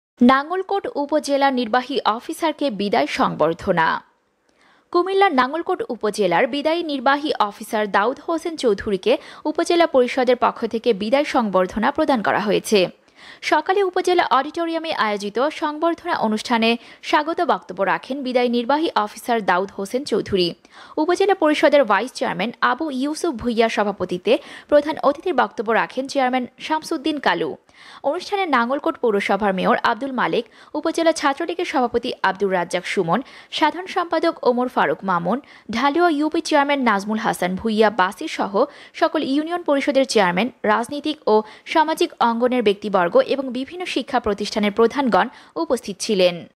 নাঙ্গলকোট উপজেলা নির্বাহী অফিসারকে বিদায় সংবর্ধনা।। ComillaNews
কুমিল্লার নাঙ্গলকোট উপজেলার বিদায়ী নির্বাহী অফিসার দাউদ হোসেন চৌধুরীকে উপজেলা পরিষদের পক্ষ থেকে বিদায় সংবর্ধনা প্রদান করা হয়েছে।
সকালে উপজেলা অডিটোরিয়ামে আয়োজিত সংবর্ধনা অনুষ্ঠানে স্বাগত বক্তব্য রাখেন বিদায়ী নির্বাহী অফিসার দাউদ হোসেন চৌধুরী।
উপজেলা পরিষদের ভাইস চেয়ারম্যান আবু ইউসুফ ভূঁইয়ার সভাপতিত্বে প্রধান অতিথির বক্তব্য রাখেন চেয়ারম্যান সামছুউদ্দিন কালু।
অনুষ্ঠানে নাঙ্গলকোট পৌরসভার মেয়র আব্দুল মালেক, উপজেলা ছাত্রলীগের সভাপতি আব্দুর রাজ্জাক সুমন, সাধারণ সম্পাদক ওমর ফারুক মামুন, ঢালুয়া ইউপি চেয়ারম্যান নাজমুল হাসান ভূঁইয়া বাছির সহ সকল ইউনিয়ন পরিষদের চেয়ারম্যান, রাজনীতিক ও সামাজিক অঙ্গণের ব্যক্তিবর্গ এবং বিভিন্ন শিক্ষা প্রতিষ্ঠানের প্রধানগণ উপস্থিত ছিলেন।